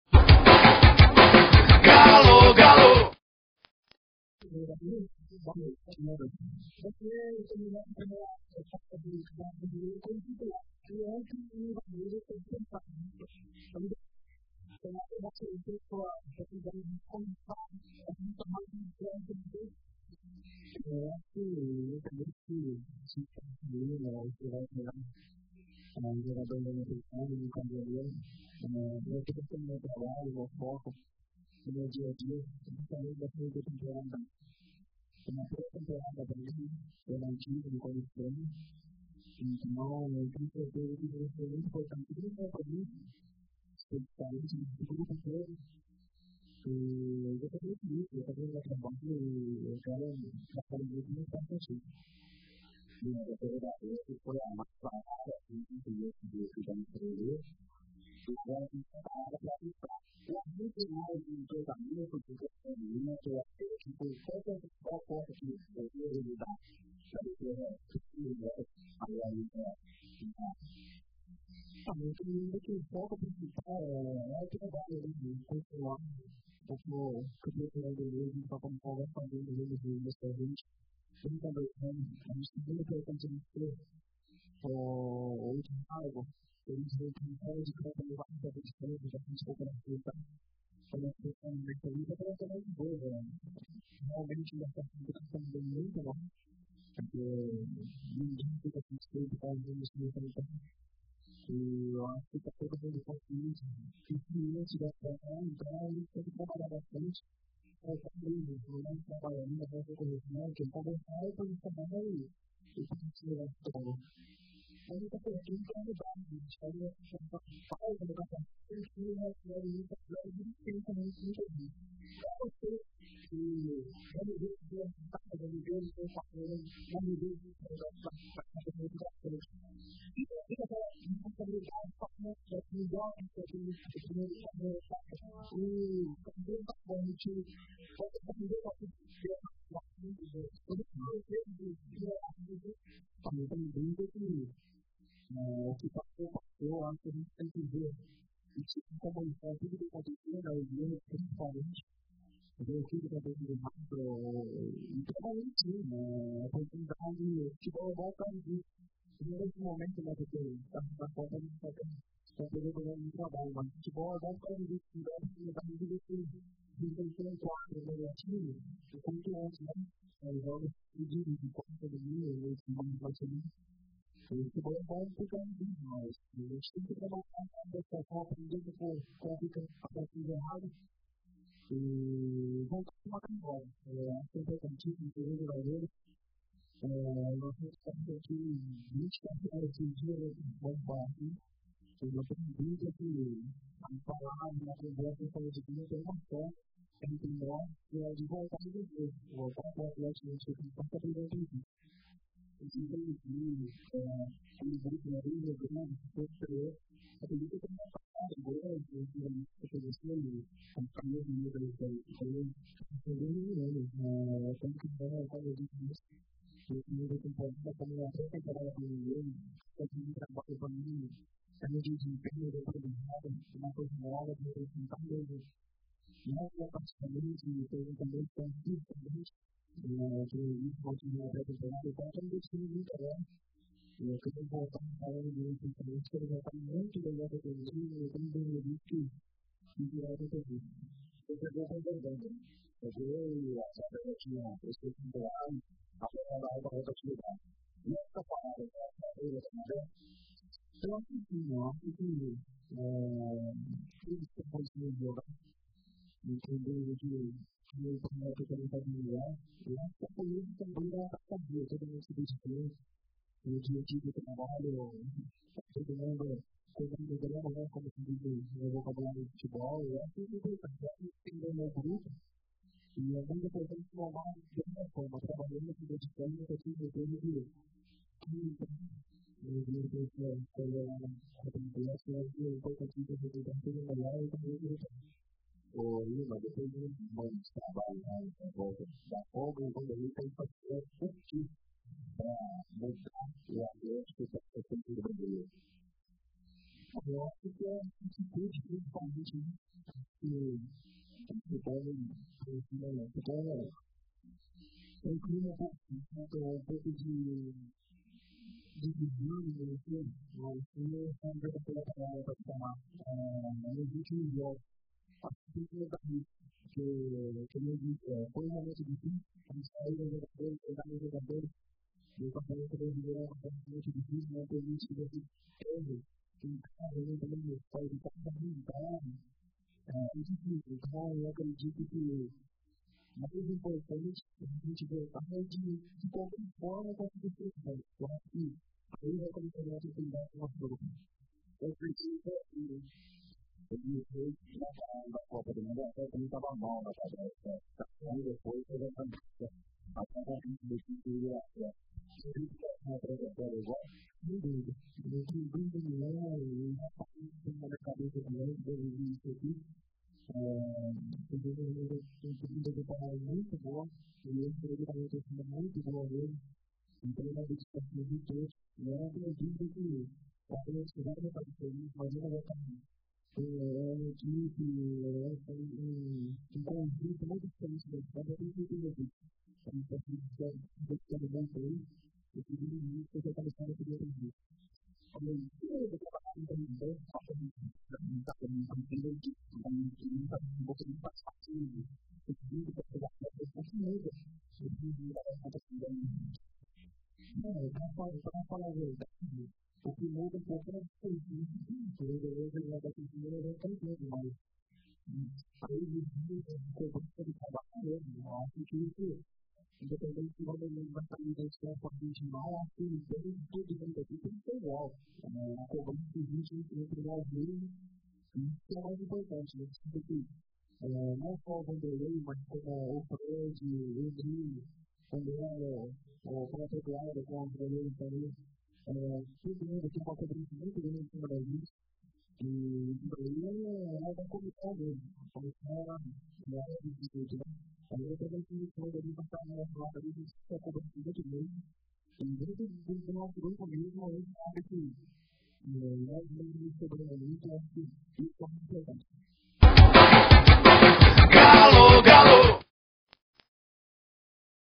Entrevista Coletiva: Lima
Confira a entrevista coletiva do zagueiro Lima.